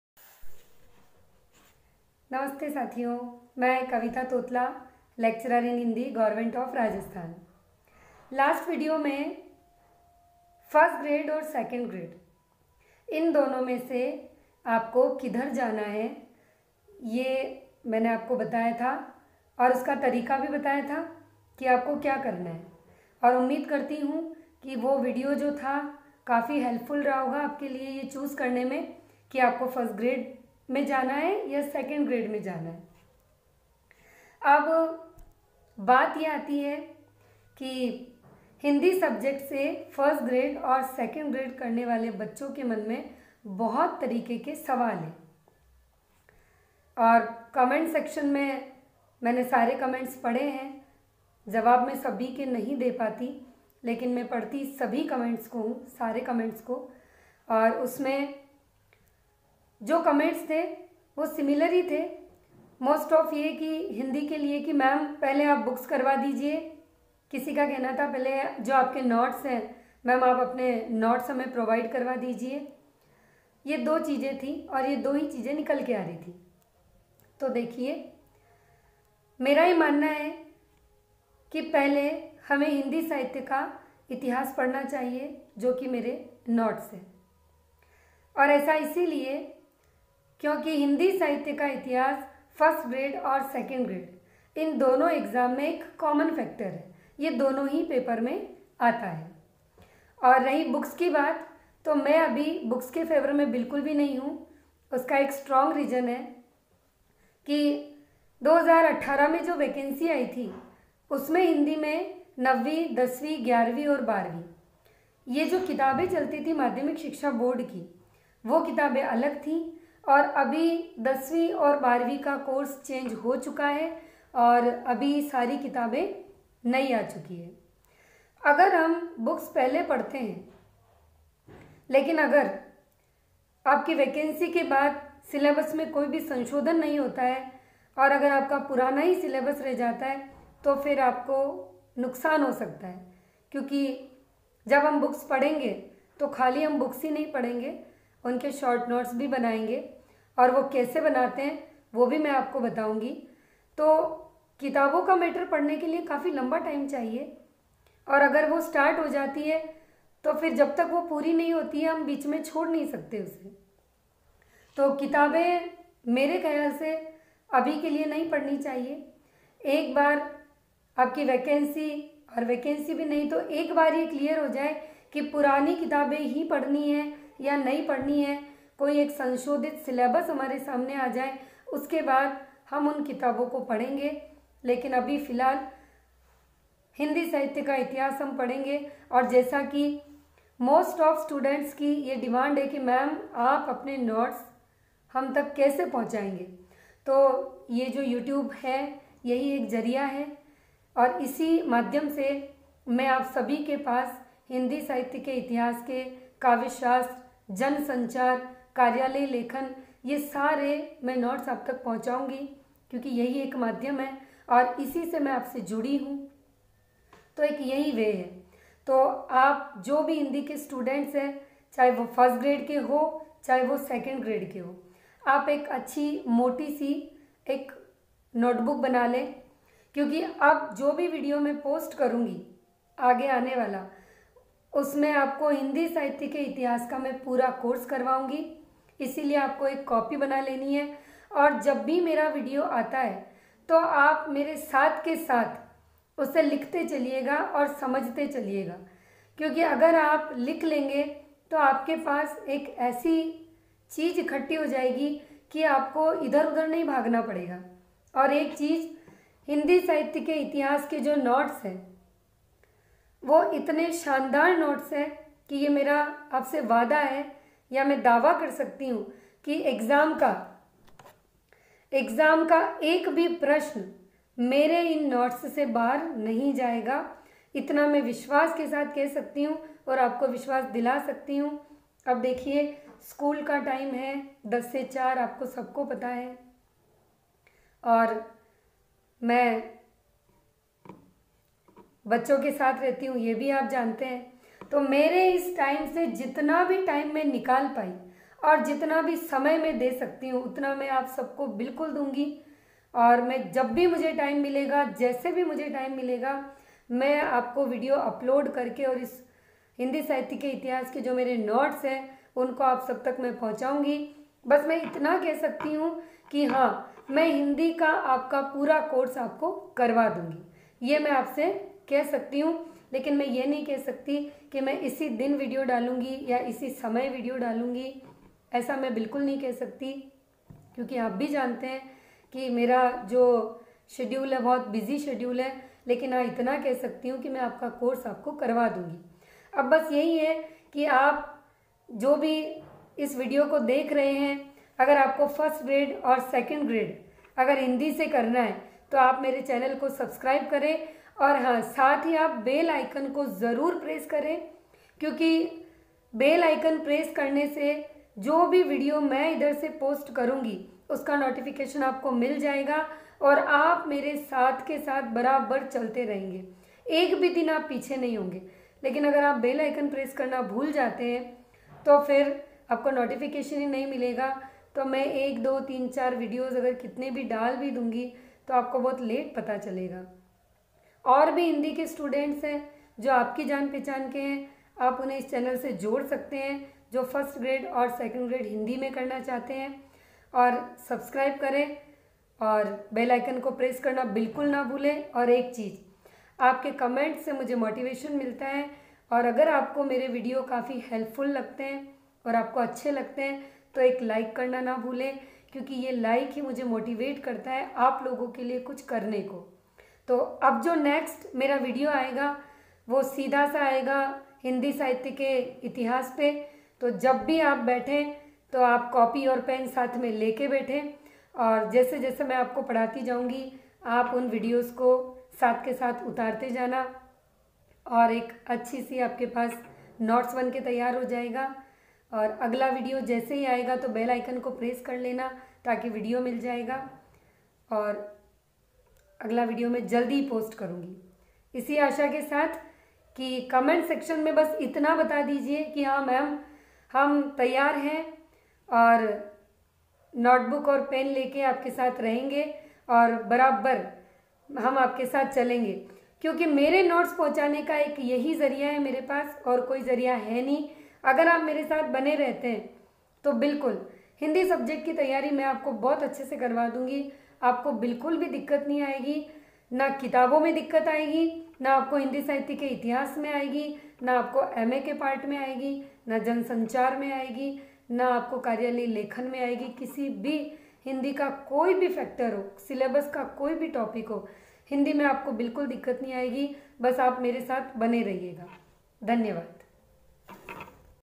RPSC first grade और second grade 👍 महत्त्वपूर्ण सूचना 👍
हिंदी साहित्य का इतिहास
प्रत्येक शनिवार या रविवार को वीडियो अपलोड किया जाएगा फर्स्ट ग्रेड और सेकंड ग्रेड के लिए
मेरे इन नोट्स से बाहर एक भी प्रश्न नहीं जाएगा तो आप साथ के साथ एक कॉपी में लिखते जाएं और नोट्स बनाते जाइएगा